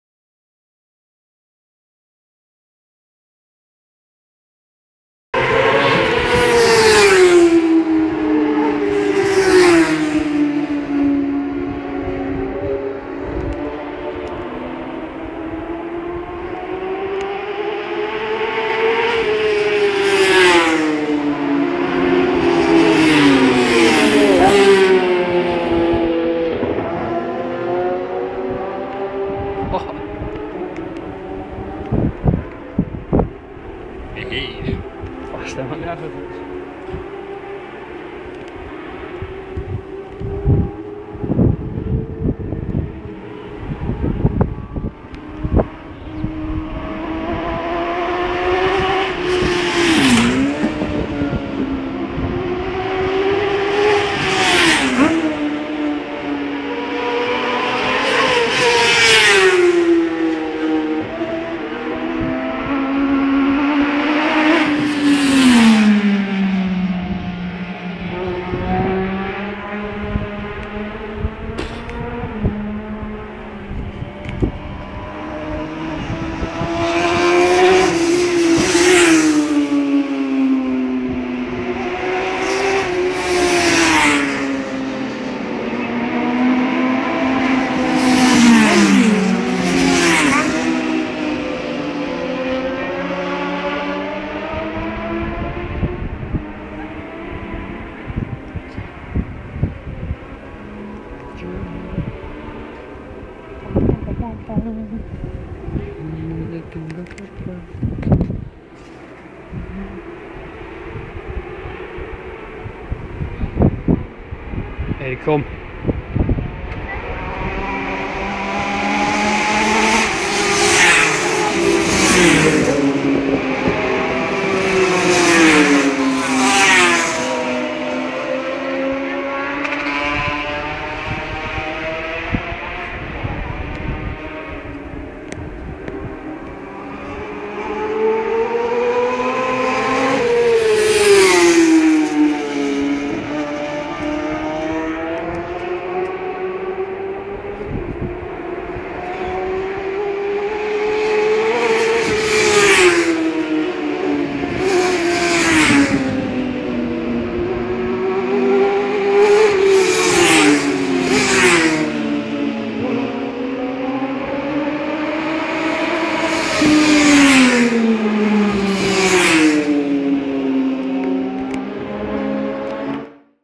Manx gran prix
practice week of the 2008 manx graprix